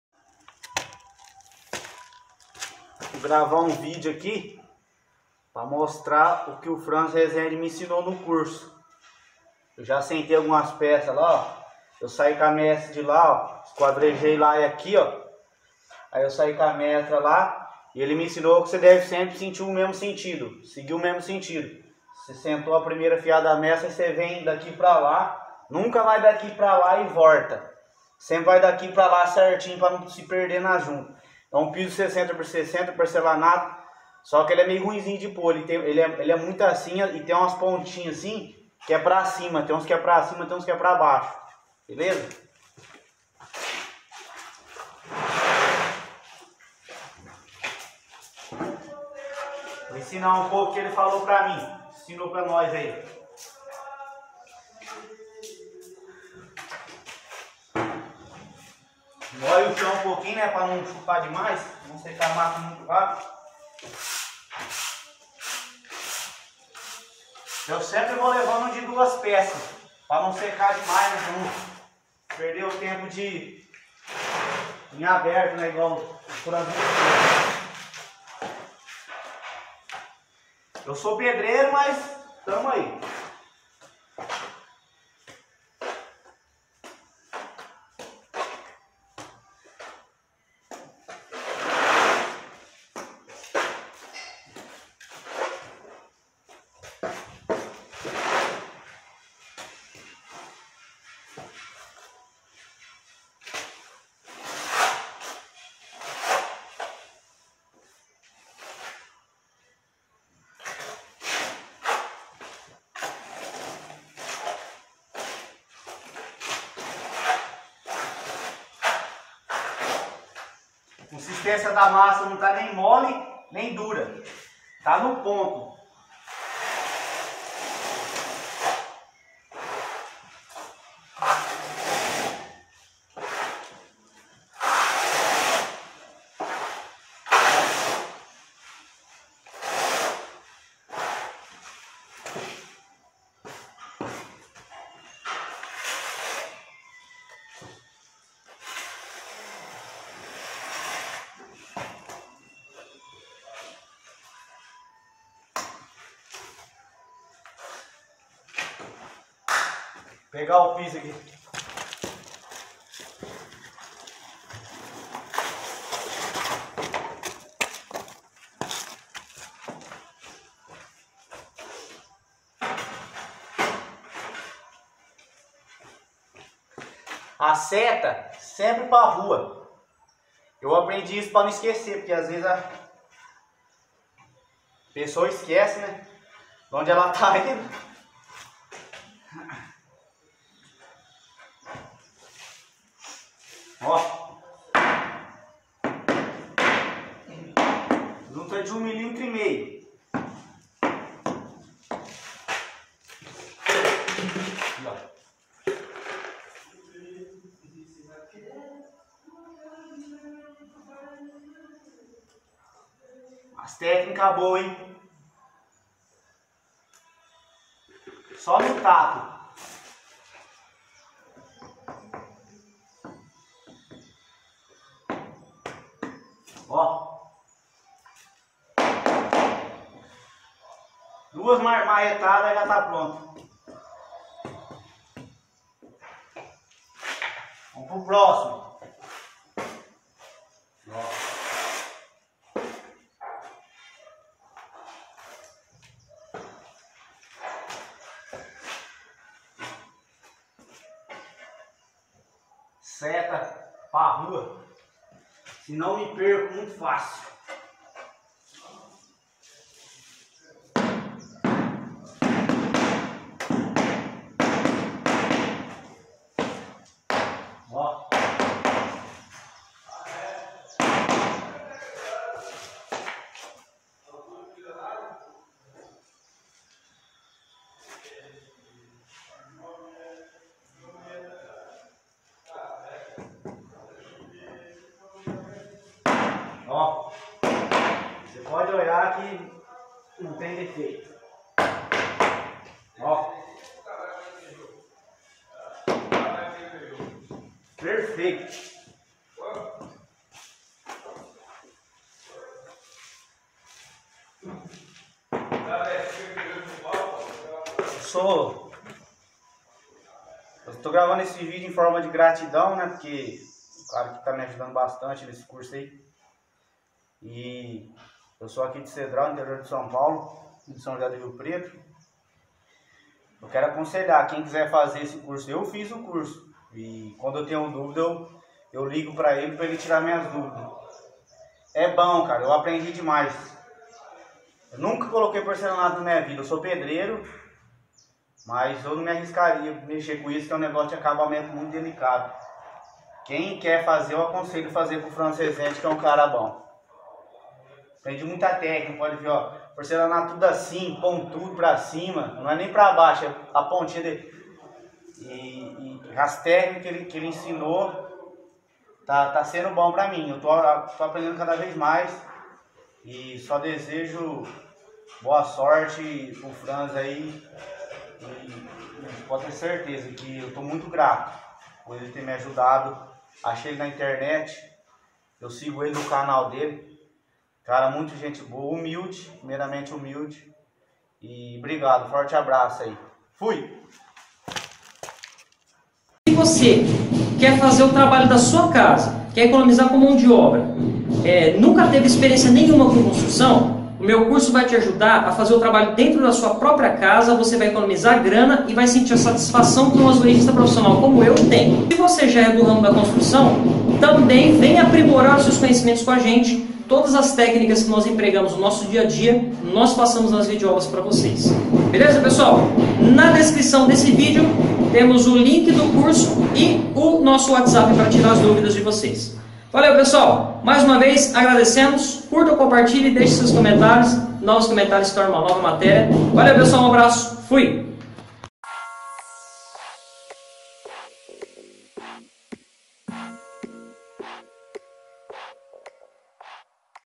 Como iniciar colocação do piso.Aluno do curso de azulejista mostra o que aprendeu.
Então faça parte do nosso grupo no Telegram.

Neste vídeo vou mostrar o depoimento do nosso aluno Elton Ferreira sobre o melhor curso de azulejista da internet-O Azulejista

Faça como ele e aprenda todas as tecnicas para colocação de pisos e revestimentos.
Tire suas dúvidas comigo no watsapp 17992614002